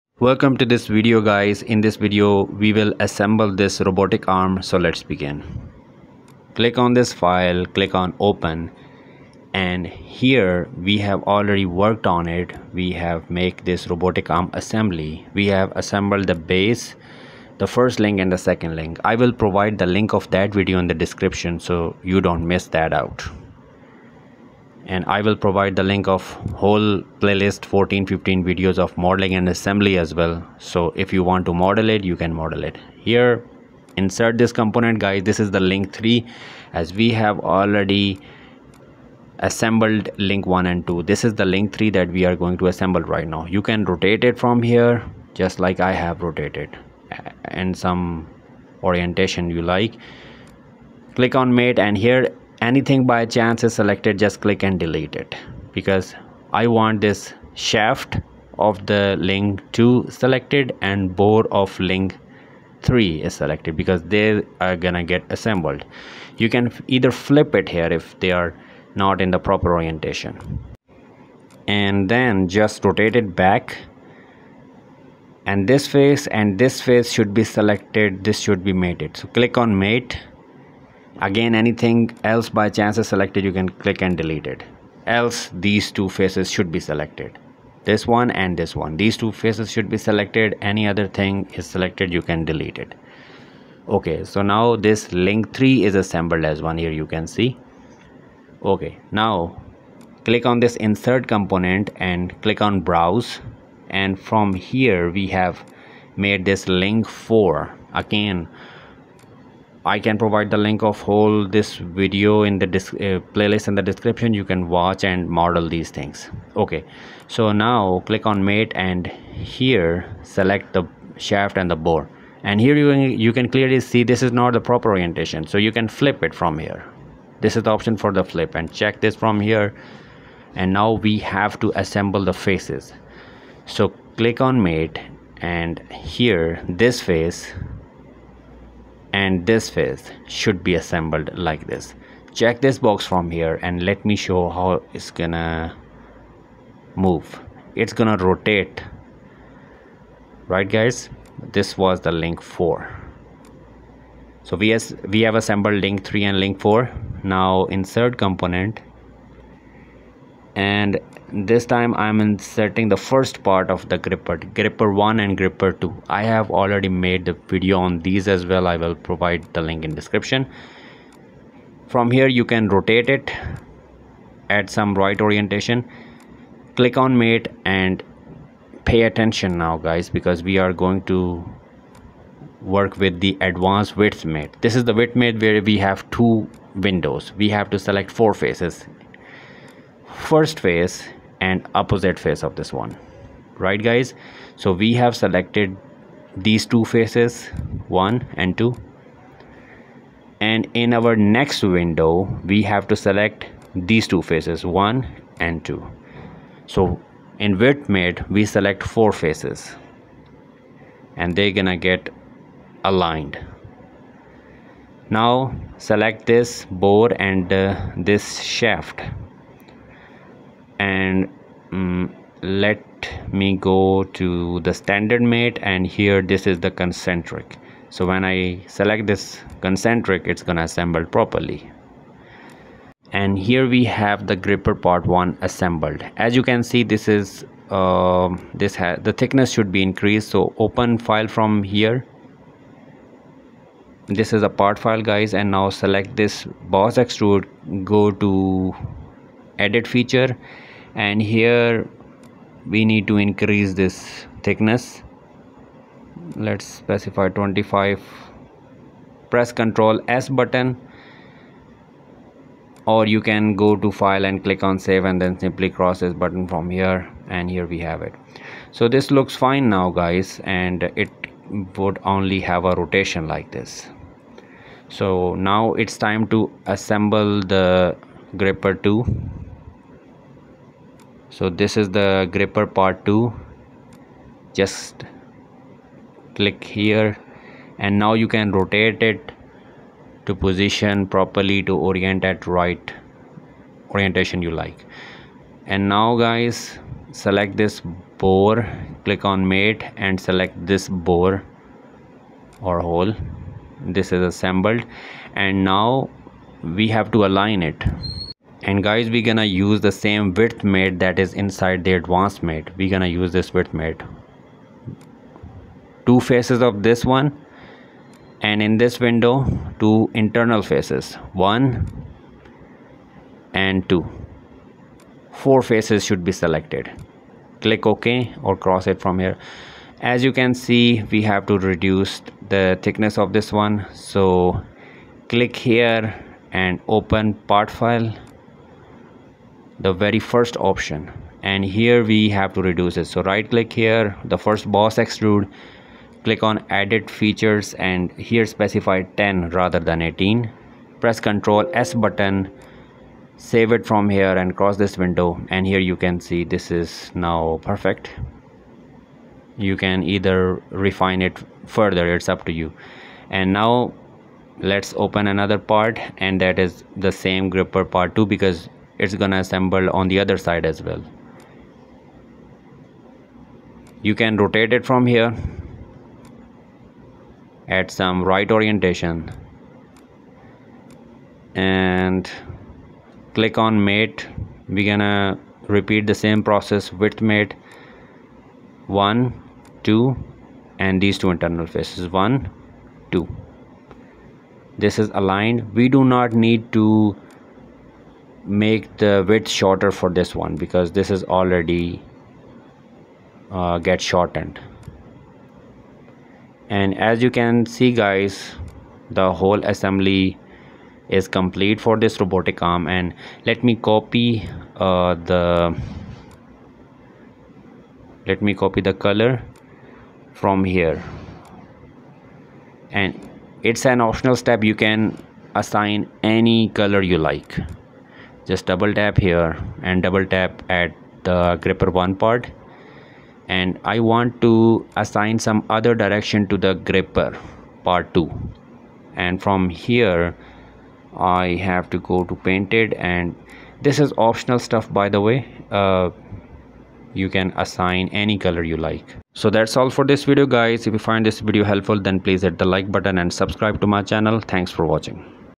Assembly of Robotic Arm in SOLIDWORKS | Robo CAD
This is Assembly of robotic arm in which i have explained how to assemble Different parts of Robotic Arm. Find the link of Previous Assembly Video below: